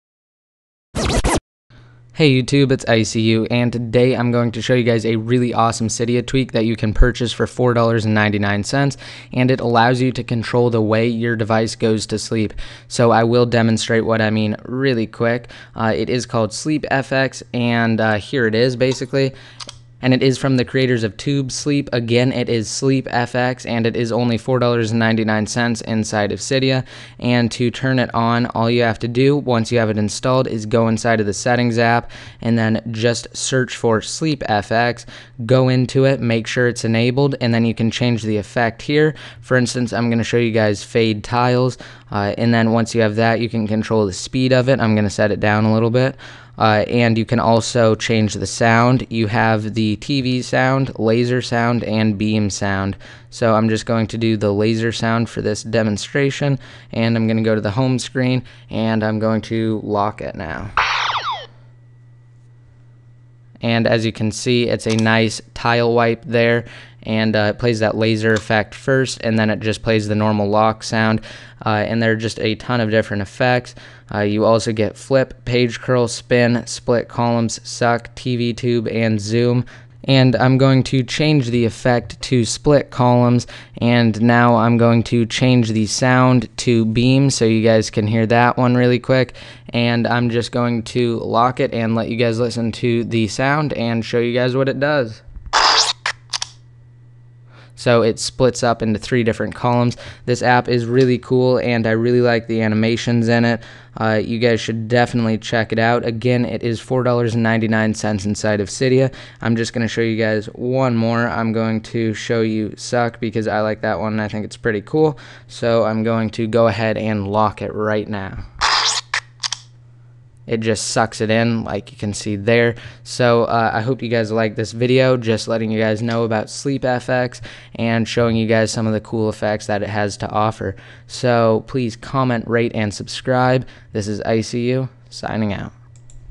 Customize The Way Your iDevice Locks - SleepFX
You can customize the way your iDevice locks with COOL effects!

$4.99 in Cydia!